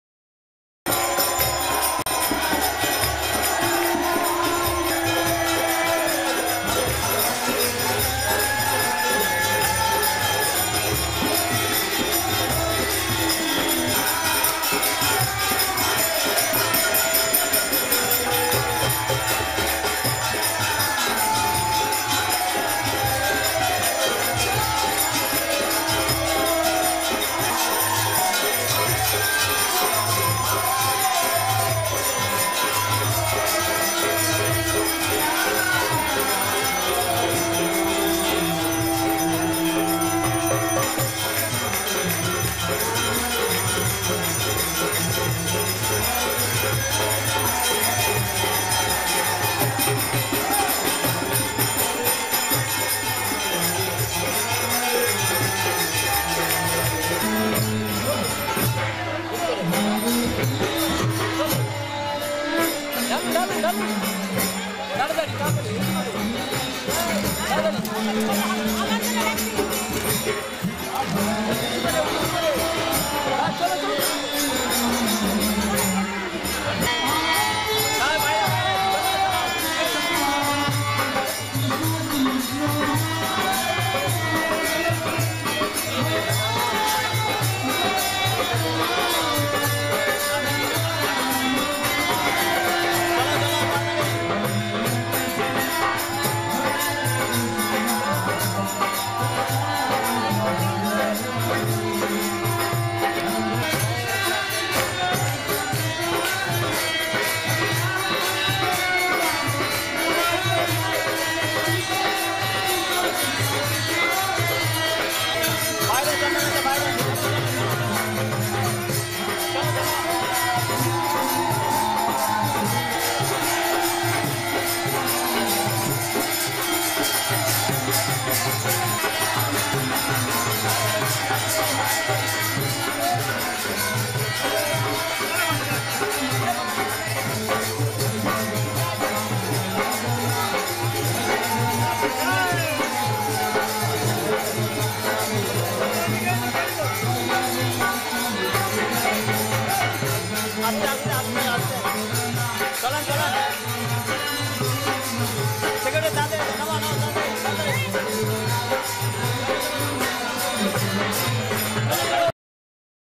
Jagannatha Snana-yatra in mayapur, Part 1
This is video from Jagannatha Snana Yatra festival, when Lord Jagannatha with His brother Baladeva and sister Subhadra are bathed in selebration of appearance this wonderful form, after this JBS are going to take bed-rest for 2 weeks before Ratha Yatra - festival of chariots, when devotees pool Them back to Vrindavan.
People came from surrounding vilagers to bathe the Lord.